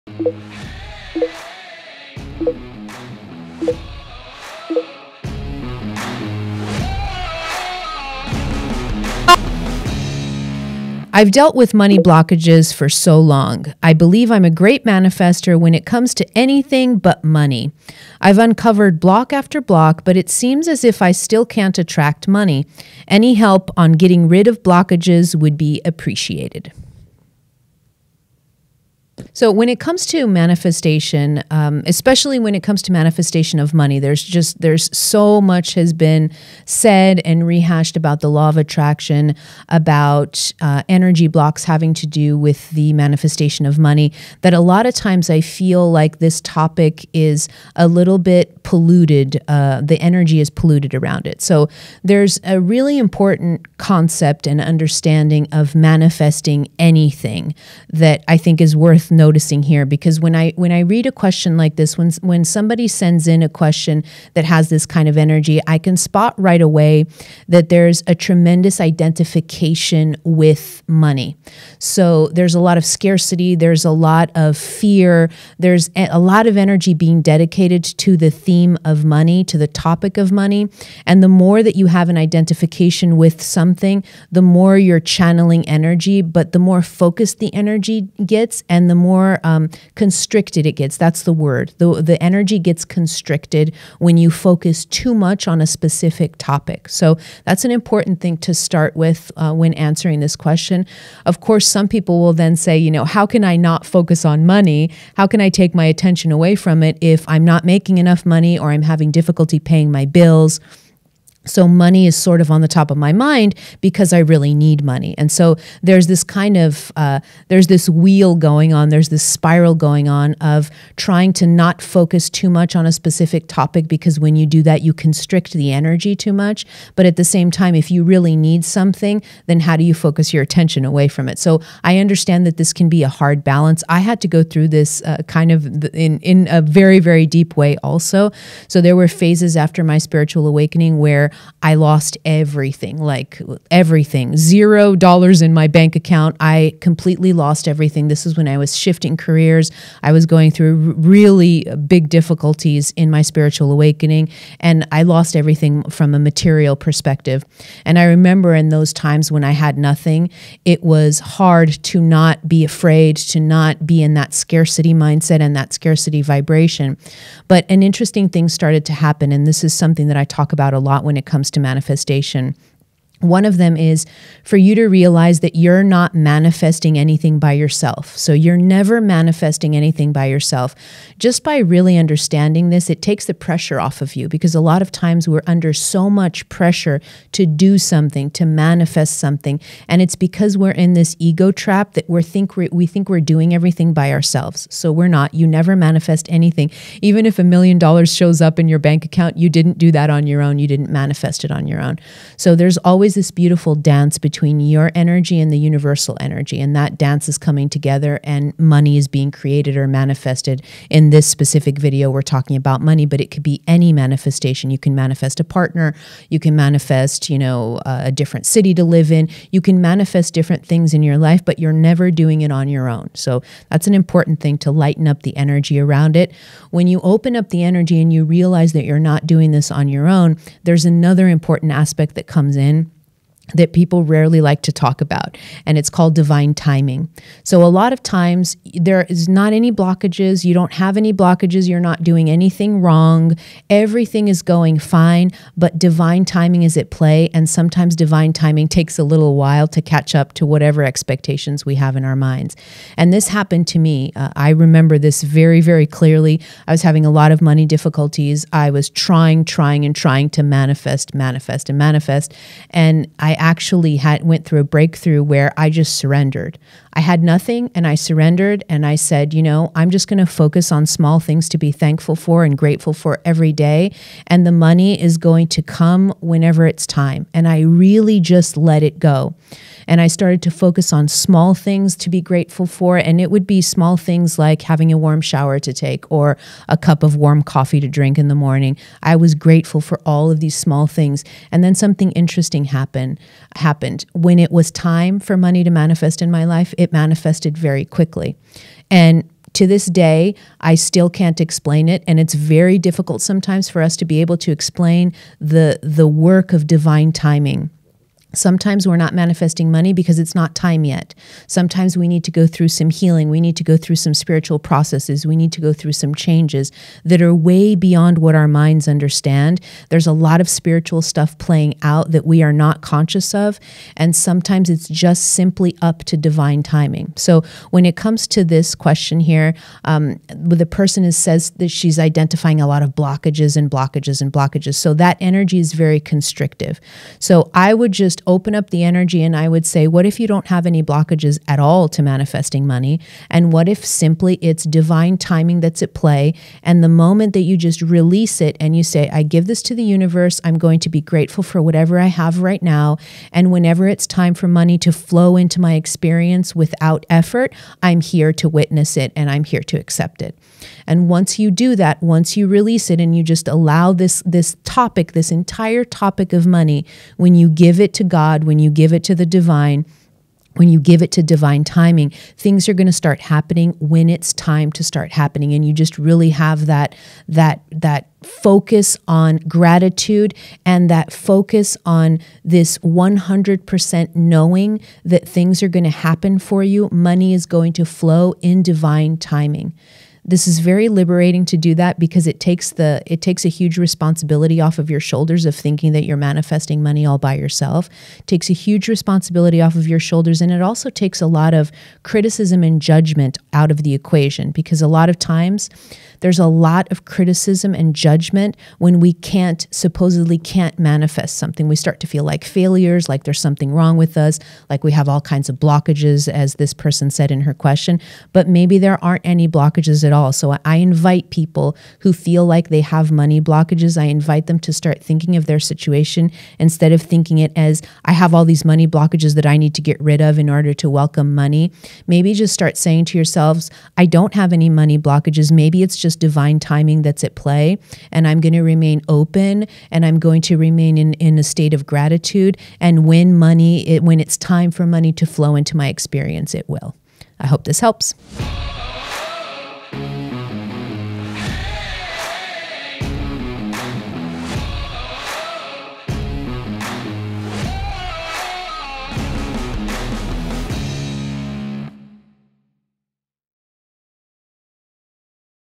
Break Free Of Money Blocks
Welcome to yet another Ask Christina! In today’s video, I dive deep into the topics of manifesting money and dissolving money blocks. I’ll share my personal story of losing everything during my spiritual awakening and what that taught me about money. You’ll hear tips on lightening your energy around finances and how to fully surrender to the universe. If you’re struggling to bring in money, it turns out you might not have a blockage after all!

FREE RESOURCES

💥 To download our popular free guided meditations or take our Heart Quiz (to discover if your heart is blocked!), head over to this

💥 Every month, I release actionable and practical videos to help you accelerate your personal growth and overcome your biggest challenges.